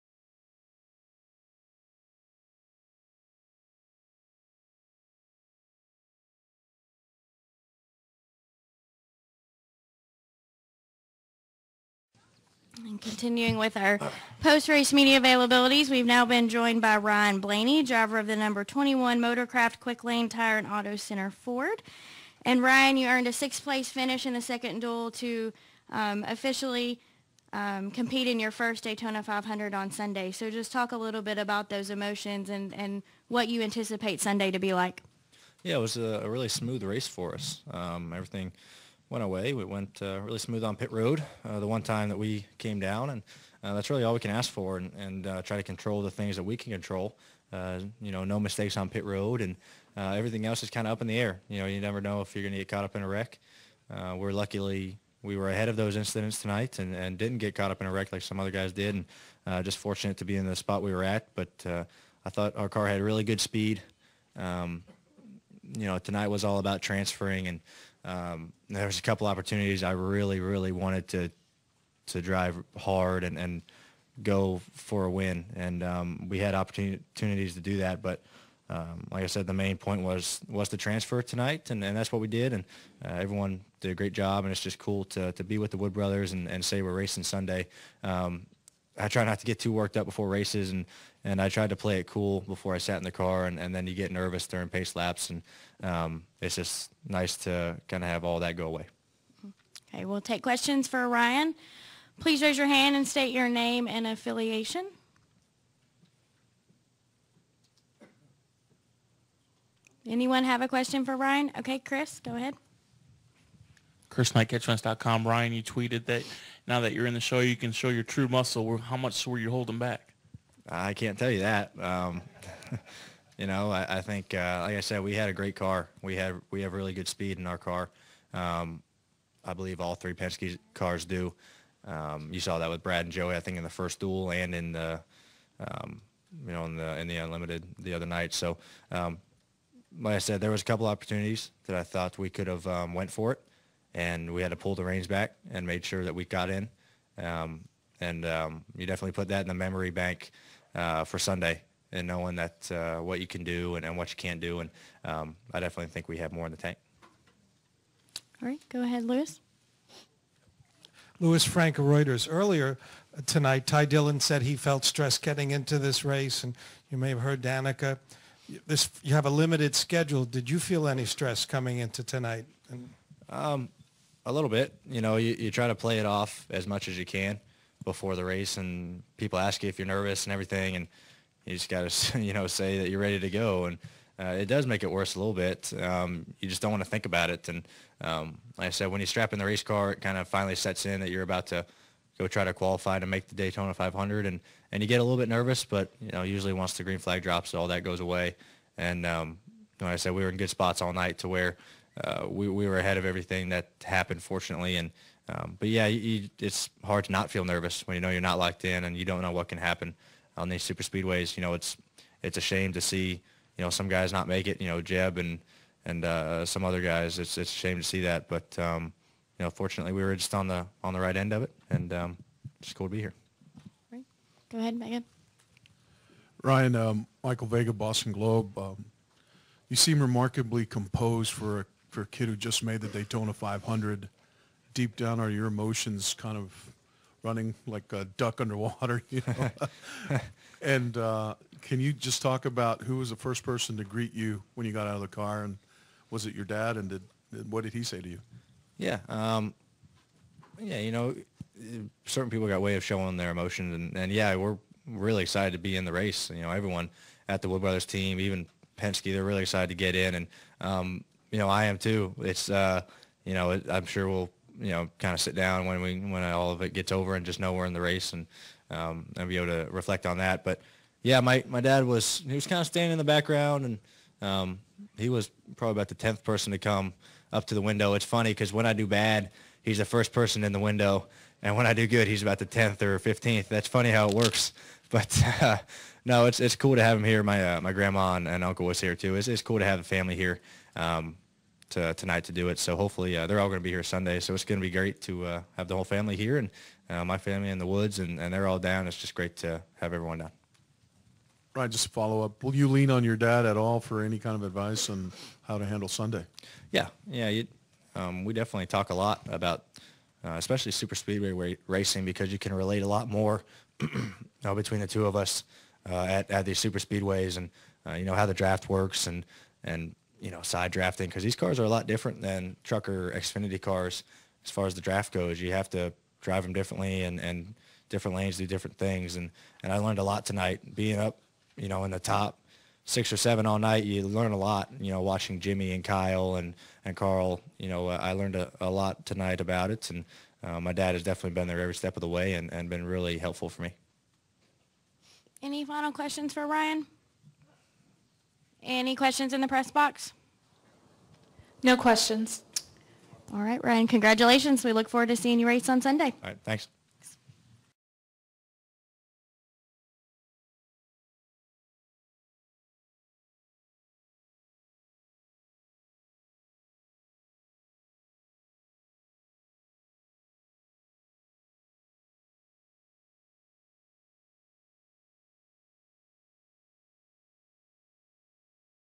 NASCAR at Daytona International Speedway Feb., 2015: Ryan Blaney post Duel
Full post Duel 2 qualifying race with Ryan Blaney from Daytona International Speedway, Thur. Feb., 19, 2015